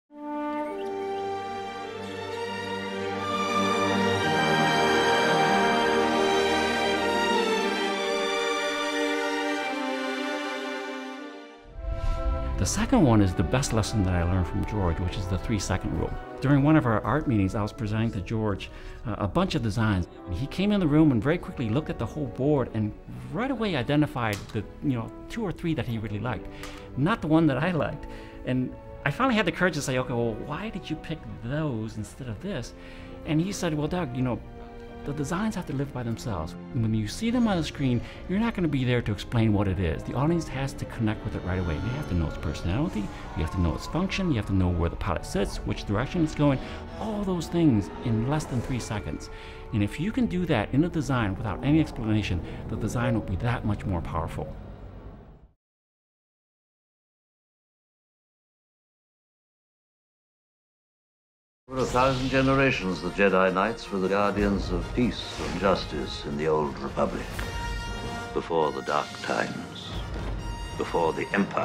Doug Chiang Looks Back - The 3-Second Rule
Design director Dough Chiang reveals one of the design principals he learned from George Lucas: when the audience sees something new on screen, they have to immediately connect with this new item. They have to understand what it is within two or three seconds.

The Star Wars Digital Movie Collection is available for the first time ever on Digital HD April 10th.